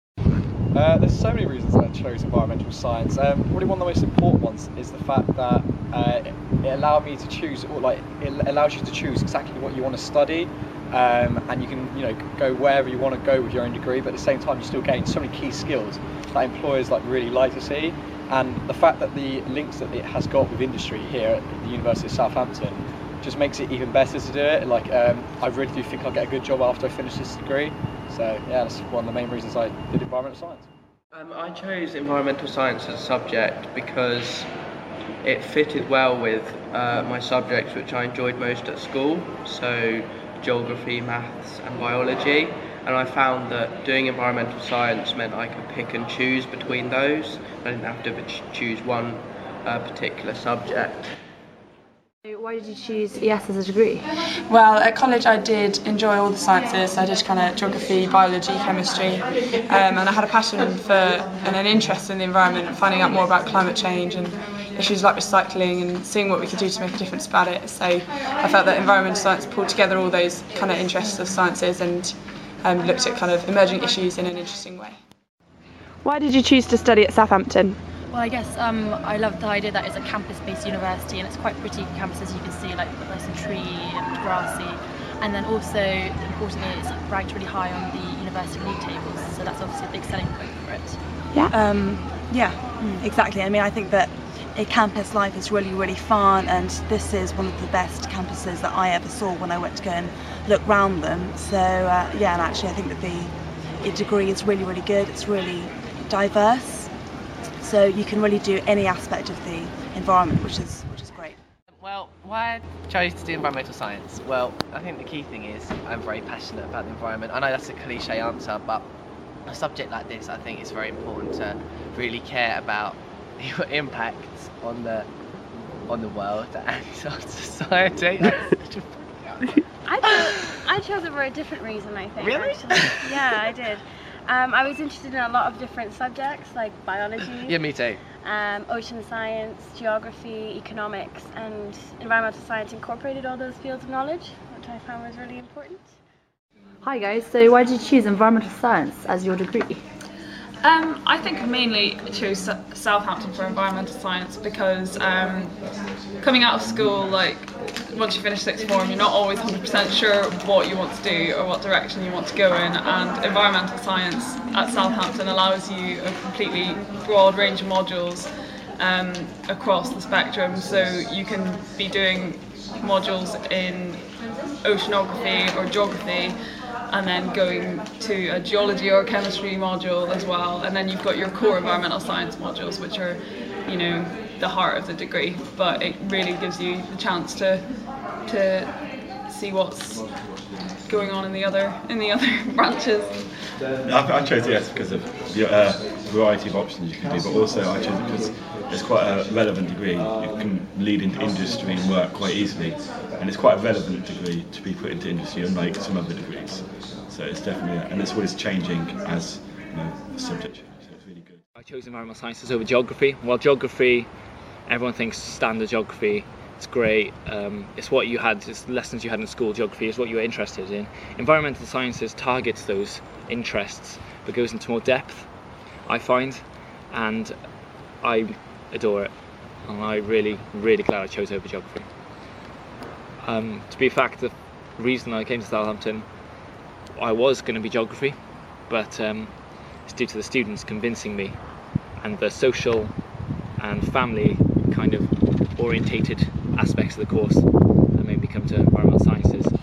Why choose to study for a degree in Environmental Sciences?
A variety of students at the University of Southampton talking about why they chose to take a degree in Environmental Sciences.